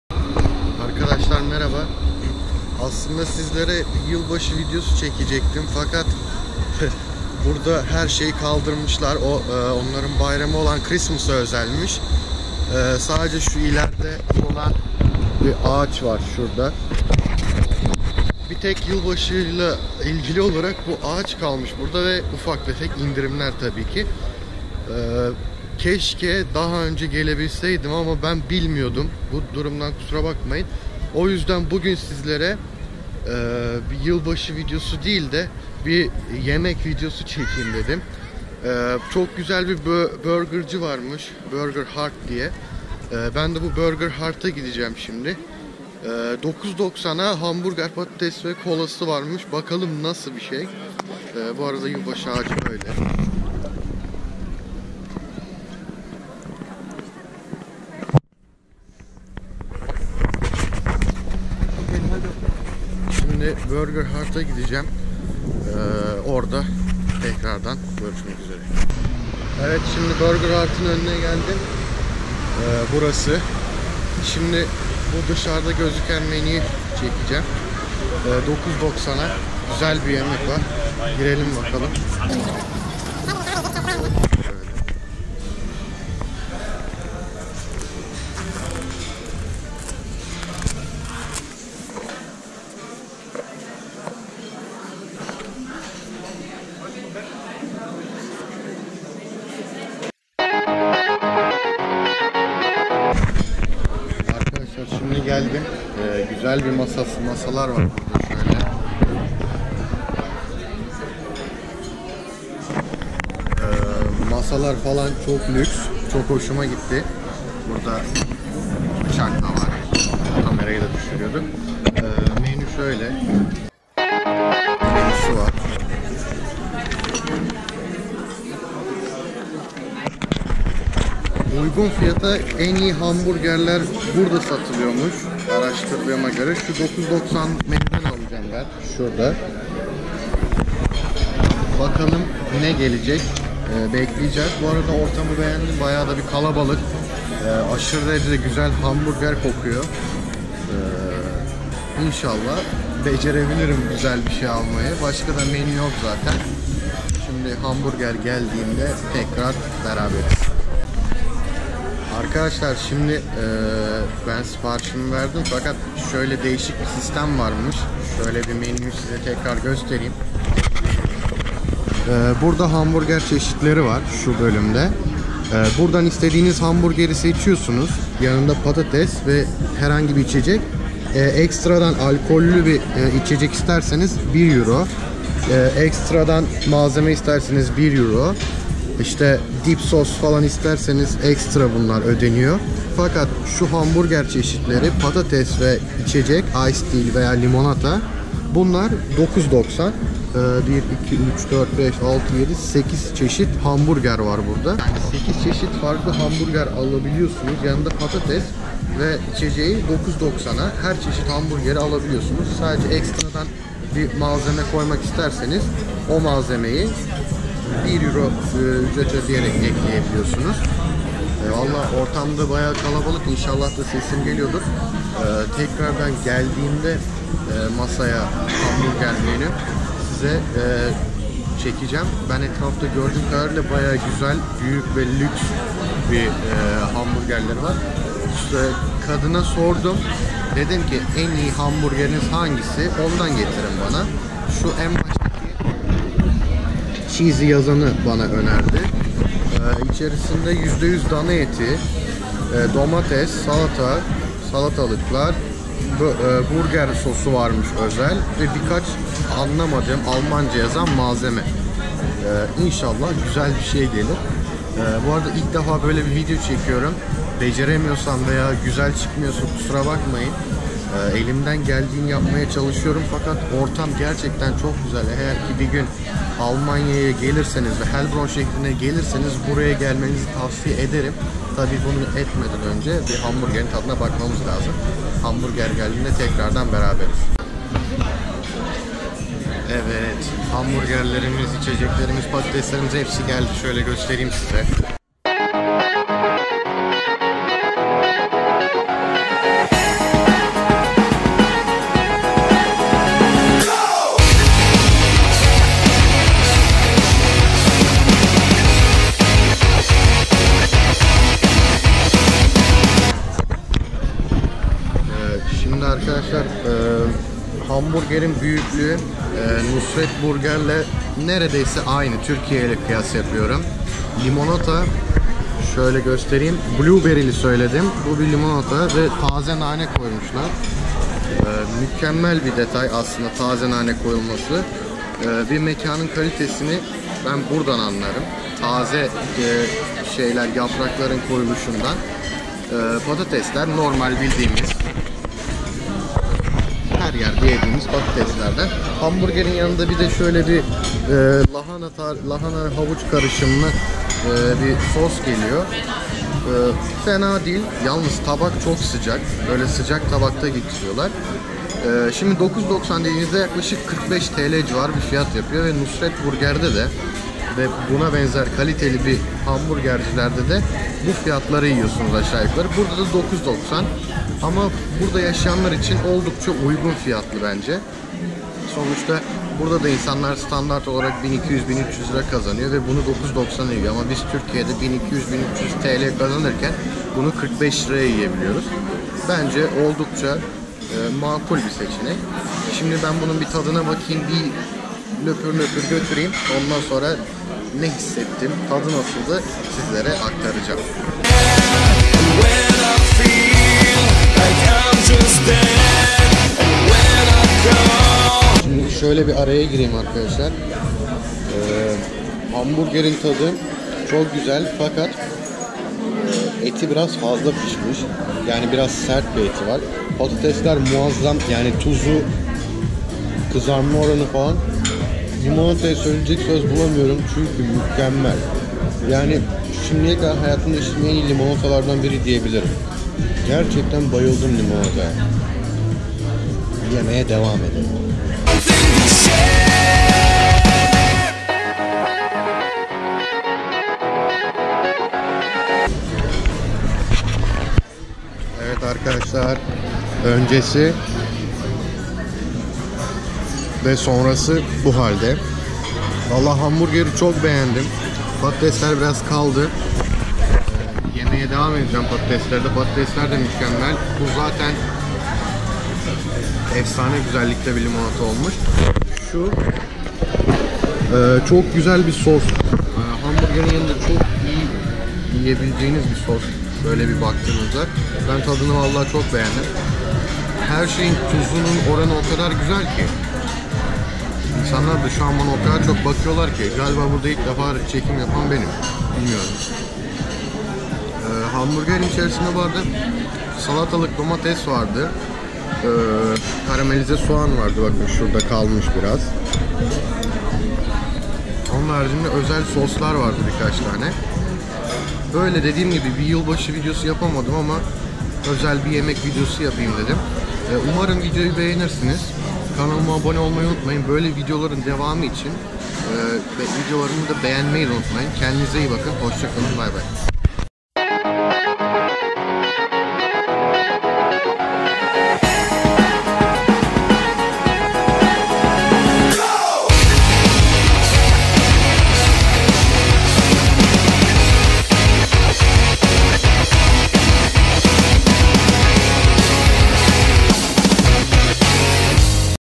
ALMANYA DA 9.90 a DEV BURGER (BurgerHEART) - Nusr-Et Burgerden iyi mi ?
Almanya da uygun fiyata yani 9.90 a yiyebileceğiniz en iyi burger menüsünün videosunu sizler için çektim.
Umarım videolarımı izlerken eğleniyorsunuzdur.